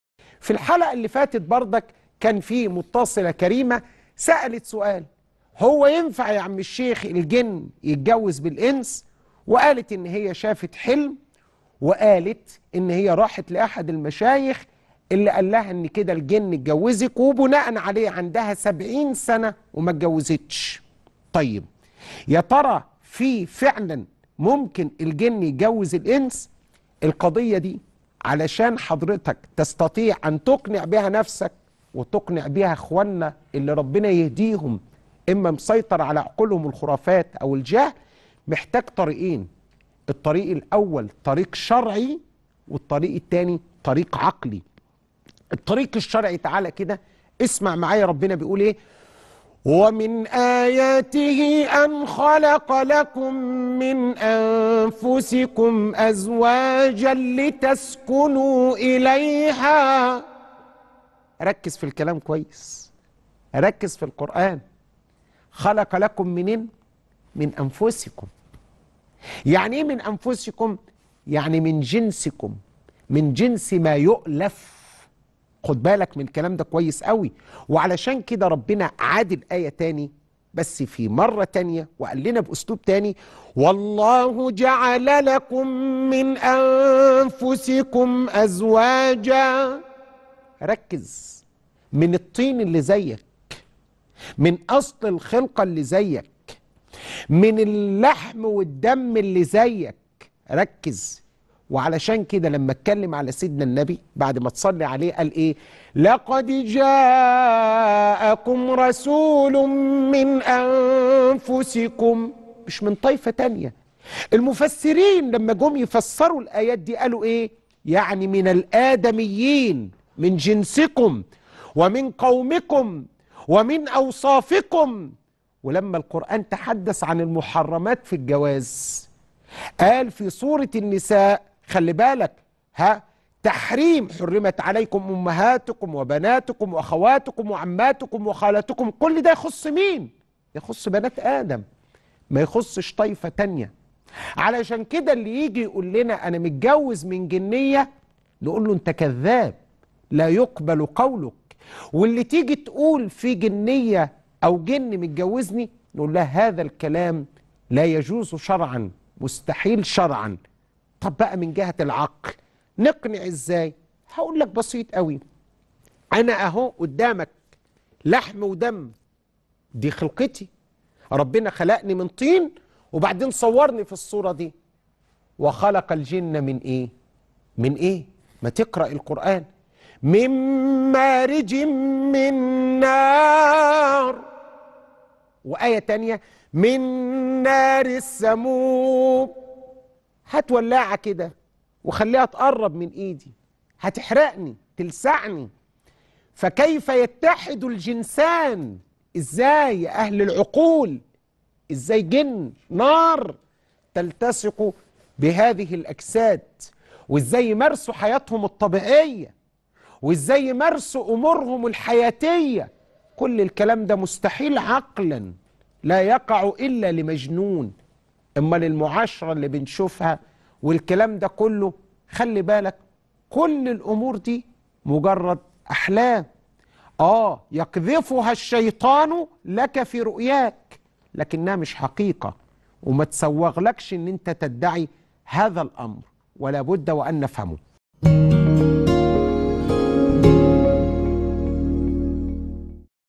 قصص مرعبة تسمعها لأول مرة من الشيخ محمد أبوبكر عن زواج الجن من الإنس
إني قريب | قصص مرعبة تسمعها لأول مرة من الشيخ محمد أبوبكر عن زواج الجن من الإنس